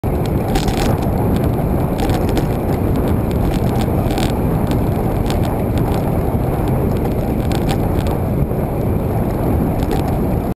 Meeting Vladamir
Vladimir was out running on   the G20.  He is in yellow and there are three minders running about 20 metres behind him.